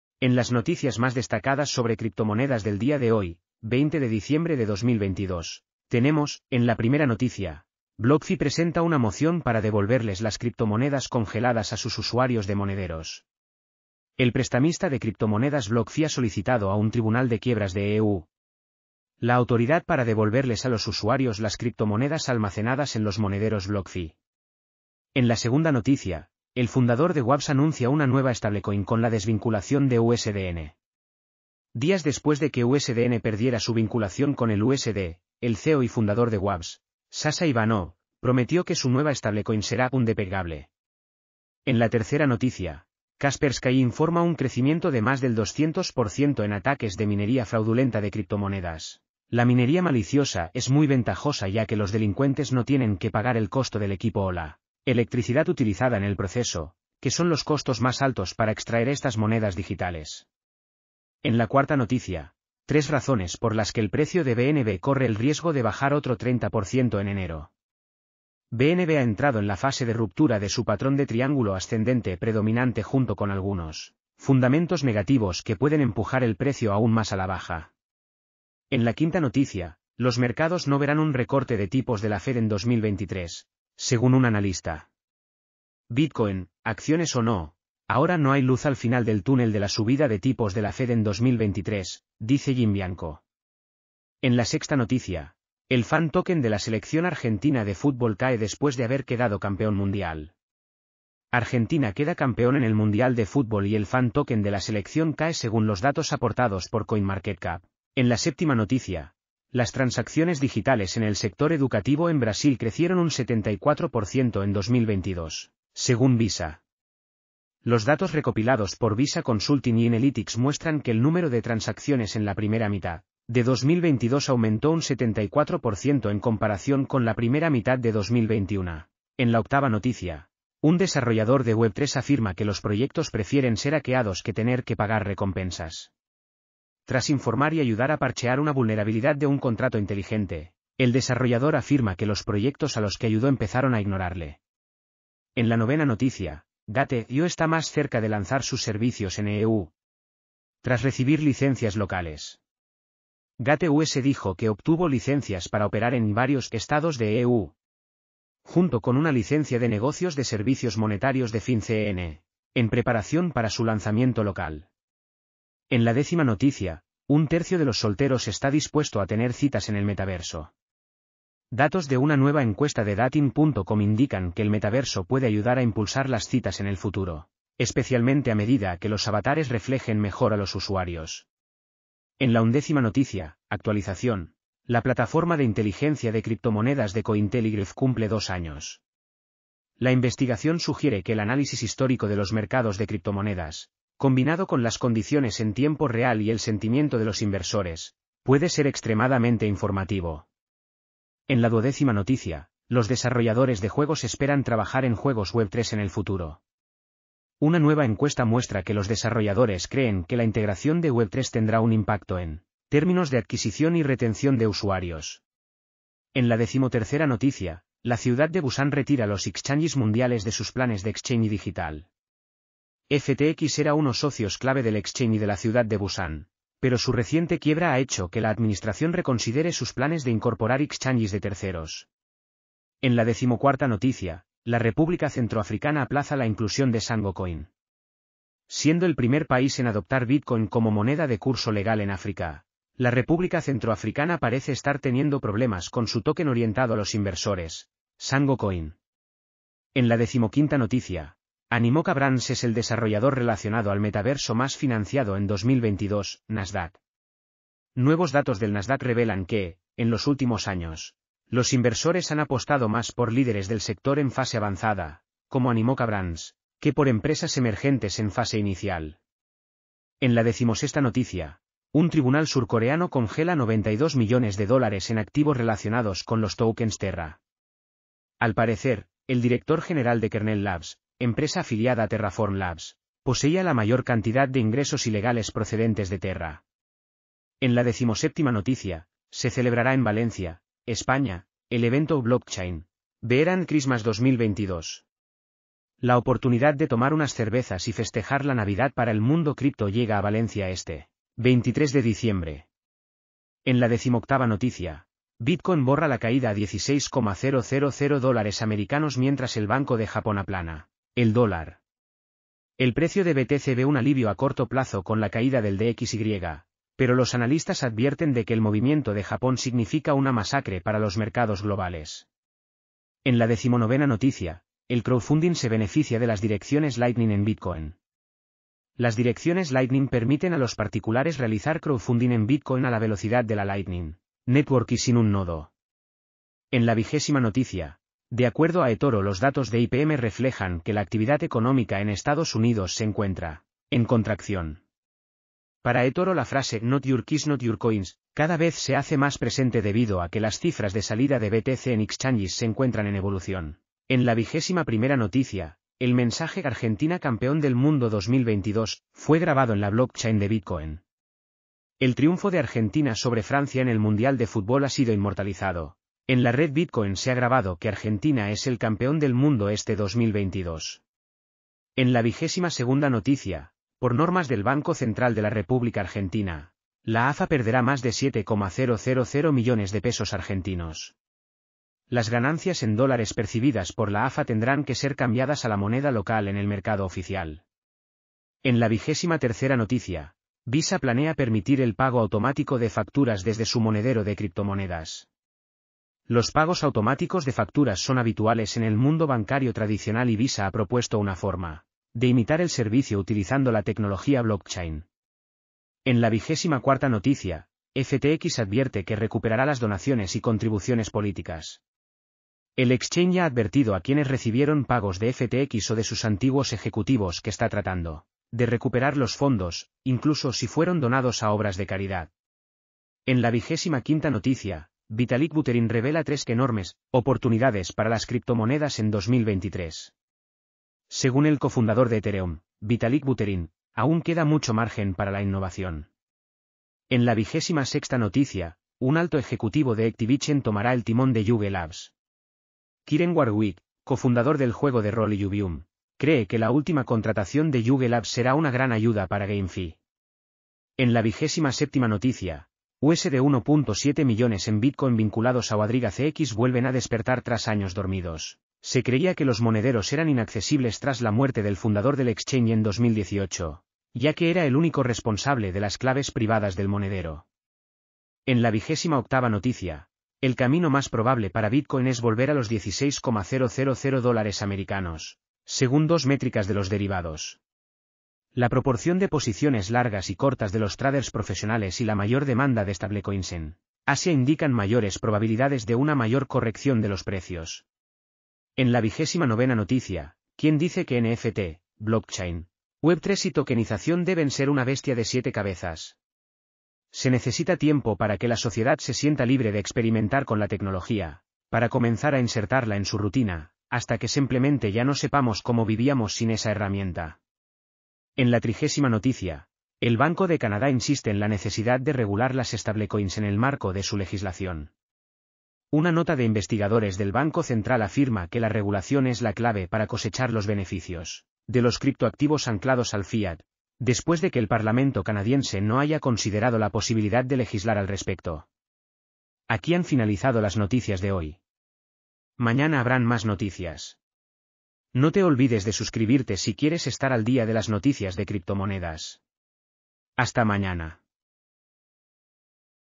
🔥 ¡BOMBAZO! | NOTICIAS Criptomonedas HOY | Cosmos | Luna | BNB
Todas las noticias destacadas del mundo de las criptomonedas de hoy, día 20 de diciembre de 2022.

⭐️  BlockFi presenta una moción para devolverles las criptomonedas congeladas a sus usuarios de monederos
🌟  El fundador de Waves anuncia una nueva stablecoin con la desvinculación de USDN
😱  Kaspersky informa un crecimiento de más del 200% en ataques de minería fraudulenta de criptomonedas
💥  3 razones por las que el precio de BNB corre el riesgo de bajar otro 30% en enero
😱  Los mercados no verán un recorte de tipos de la Fed en 2023, según un analista
➡️  El Fan Token de la Selección Argentina de Fútbol cae después de haber quedado campeón mundial
🥇  Las transacciones digitales en el sector educativo en Brasil crecieron un 74% en 2022, según Visa
🌟  Un desarrollador de Web3 afirma que los proyectos prefieren ser hackeados que tener que pagar recompensas
🌟  Gate.io está más cerca de lanzar sus servicios en EE.UU. tras recibir licencias locales
🏆  Un tercio de los solteros está dispuesto a tener citas en el metaverso
⚫️  Actualización: la plataforma de inteligencia de criptomonedas de Cointelegraph cumple 2 años
▶️  Los desarrolladores de juegos esperan trabajar en juegos Web3 en el futuro
➡️  La ciudad de Busan retira los exchanges mundiales de sus planes de exchange digital
🌟  La República Centroafricana aplaza la inclusión de Sango Coin
✅  Animoca Brands es el desarrollador relacionado al metaverso más financiado en 2022: Nasdaq
⚫️  Un tribunal surcoreano congela 92 millones de dólares en activos relacionados con los tokens Terra
✔️  Se celebrará en Valencia, España, el evento Blockchain, beer and Christmas 2022
💡  Bitcoin borra la caída a USD 16,000 mientras el Banco de Japón aplana el dólar
➡️  El crowdfunding se beneficia de las direcciones Lightning en Bitcoin
🥇  De acuerdo a eToro los datos de IPM reflejan que la actividad económica en Estados Unidos se encuentra en contracción
🔴  El mensaje Argentina Campeón del mundo 2022 fue grabado en la Blockchain de Bitcoin
🥇  Por normas del Banco Central de la República Argentina, la AFA perderá más de 7,000 millones de pesos argentinos
😲  Visa planea permitir el pago automático de facturas desde su monedero de criptomonedas
✅  FTX advierte que recuperará las donaciones y contribuciones políticas
▶️  Vitalik Buterin revela 3 enormes oportunidades para las criptomonedas en 2023
🟢  Un alto ejecutivo de Activision tomará el timón de Yuga Labs
➡️  USD 1.7 millones en Bitcoin vinculados a QuadrigaCX vuelven a despertar tras años dormidos
💥  El camino más probable para Bitcoin es volver a los USD 16,000, según 2 métricas de los derivados
🥇  ¿Quién dice que NFT, blockchain, web3 y tokenización deben ser una bestia de 7 cabezas?
👉🏼  El Banco de Canadá insiste en la necesidad de regular las stablecoins en el marco de su legislación

¡Esperamos que te haya gustado el video y las noticias crypto de hoy!